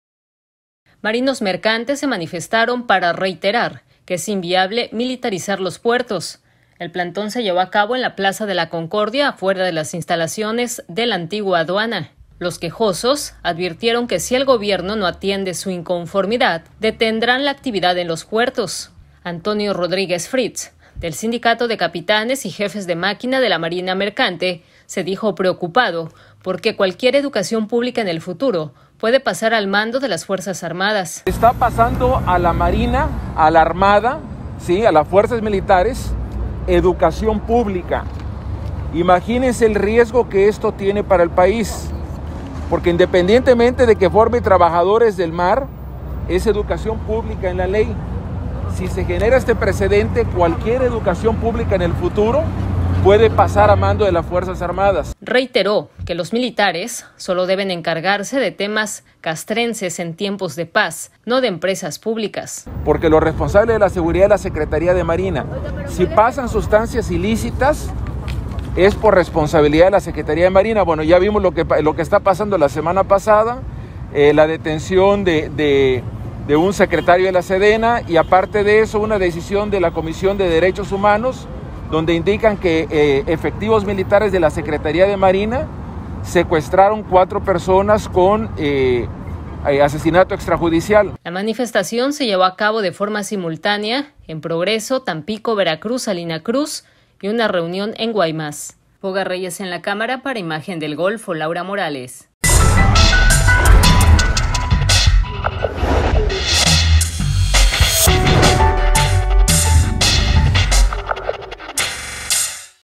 Marinos de Veracruz protestan contra militarización del puerto
Marinos mercantes se manifestaron para reiterar que es inviable militarizar los puertos, advirtieron que si el gobierno no atiende su inconformidad, detendrán la actividad portuaria.